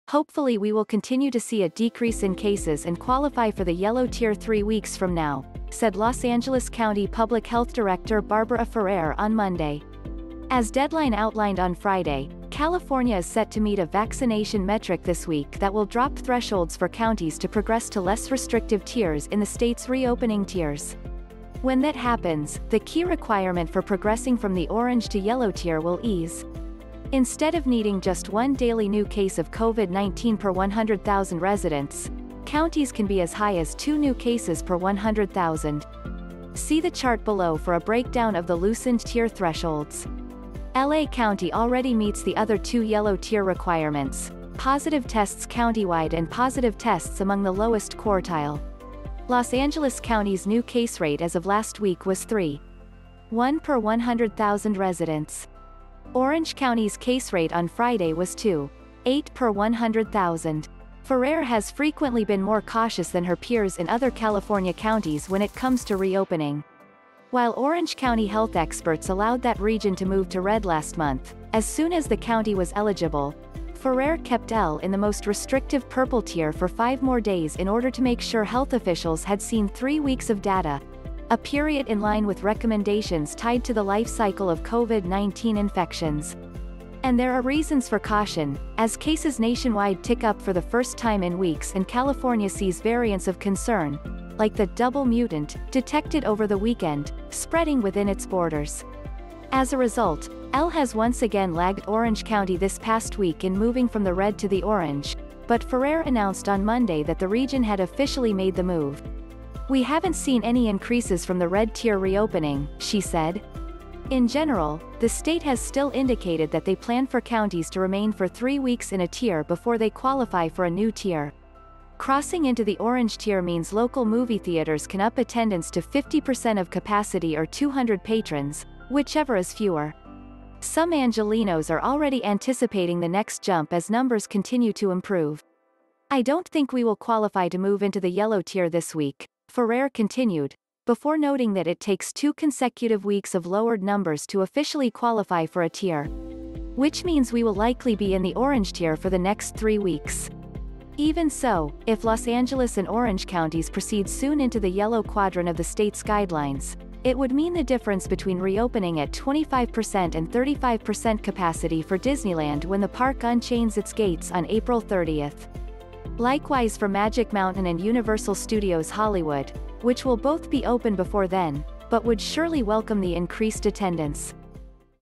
Top Los Angeles Health Official Sees County In Yellow Reopening Tier By
“Hopefully we will continue to see a decrease in cases and qualify for the Yellow tier three weeks from now, ” said Los Angeles County Public Health Director Barbara Ferrer on Monday. As Deadline outlined on Friday, California is set to meet a vaccination metric this week that will drop thresholds for counties to progress to less restrictive tiers in the state’s reopening tiers. When that happens, the key requirement for progressing from the Orange to Yellow tier will ease. Instead of needing just 1 daily new case of Covid-19 per 100, 000 residents, counties can be as high as 2 new cases per 100, 000. See the chart below for a breakdown of the loosened tier thresholds. L. A. County already meets the other two Yellow-tier requirements — positive tests countywide and positive tests among the lowest quartile. Los Angeles County’s new case rate as of last week was 3. 1 per 100, 000 residents. Orange County’s case rate on Friday was 2. 8 per 100, 000. Ferrer has frequently been more cautious than her peers in other California counties when it comes to reopening. While Orange County health experts allowed that region to move to Red last month, as soon as the county was eligible, Ferrer kept L. in the most restrictive Purple tier for five more days in order to make sure health officials had seen three weeks of data — a period in line with recommendations tied to the life cycle of Covid-19 infections. And there are reasons for caution, as cases nationwide tick up for the first time in weeks and California sees variants of concern — like the “double mutant” detected over the weekend — spreading within its borders. As a result, L. has once again lagged Orange County this past week in moving from the Red to the Orange, but Ferrer announced on Monday that the region had officially made the move.“We haven’t seen any increases from the Red tier reopening, ” she said. “In general, the state has still indicated that they plan for counties to remain for three weeks in a tier before they qualify for a new tier. ”Crossing into the Orange tier means local movie theaters can up attendance to 50% of capacity or 200 patrons, whichever is fewer. Some Angelenos are already anticipating the next jump as numbers continue to improve.“I don’t think we will qualify to move into the Yellow tier this week, ” Ferrer continued, before noting that it takes two consecutive weeks of lowered numbers to officially qualify for a tier. “Which means we will likely be in the Orange tier for the next three weeks. ”Even so, if Los Angeles and Orange Counties proceed soon into the Yellow quadrant of the state’s guidelines, it would mean the difference between reopening at 25% and 35% capacity for Disneyland when the park unchains its gates on April 30. Likewise for Magic Mountain and Universal Studios Hollywood, which will both be open before then, but would surely welcome the increased attendance.